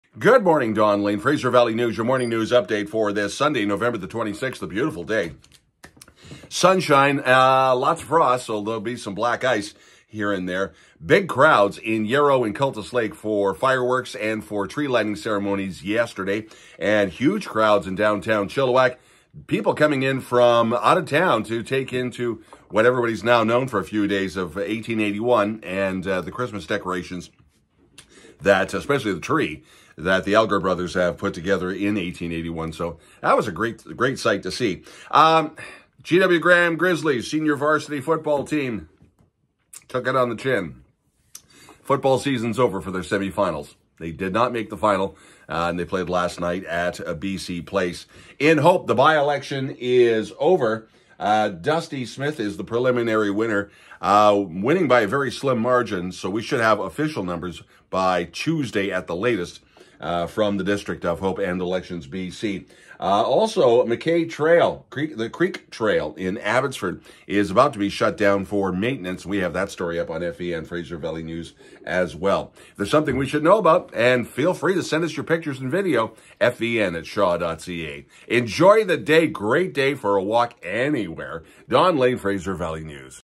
FVN AM News Sunday November 26, 2023. Hope By-Election , Xmas Lights in Yarrow and Cultus Lake
Hope By-Election , Xmas Lights in Yarrow and Cultus Lake